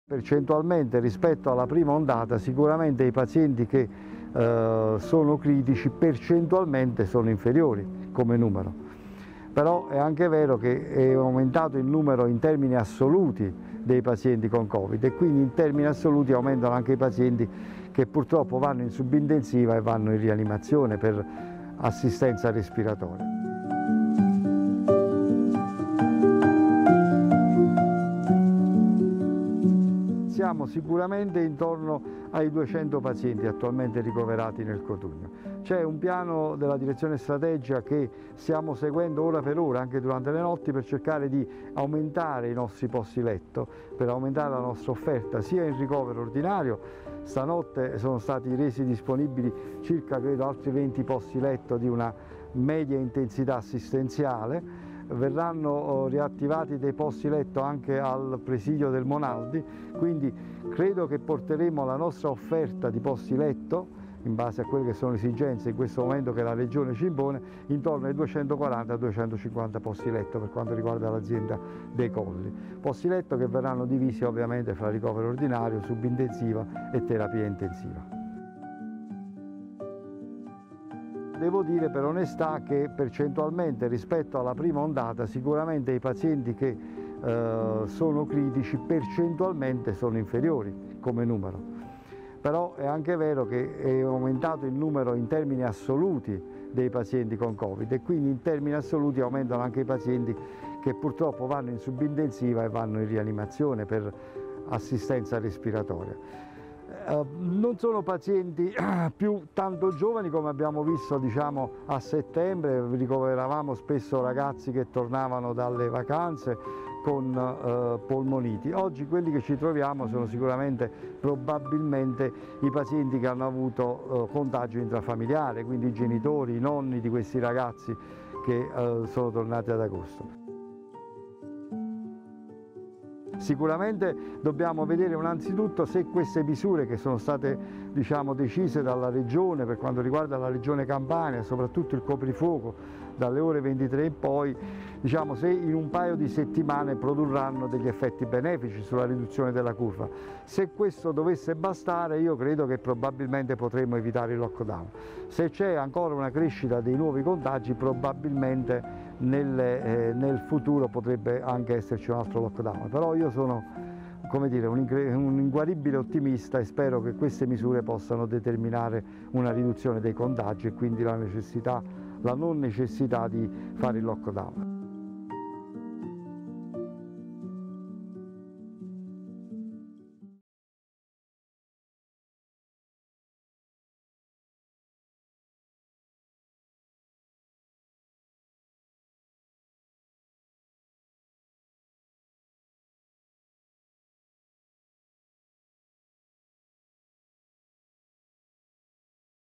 L'infettivologo del Cotugno: "Se le restrizioni funzionano è possibile evitare lockdown totale"
Il dottor Rodolfo Punzi, infettivologo dell'ospedale Cotugno da sempre in prima linea nella lotta al Covid, spiega che attualmente nel nosocomio ci sono circa 200 pazienti. La situazione non è semplice e rispetto alla prima ondata i pazienti gravi sono diminuiti ma il numero assoluto di positivi in Campania è aumentato. Per questo sarà fondamentale capire se le restrizioni attuate dal Governo e dal Regione Campania potranno scongiurare un nuovo lockdown.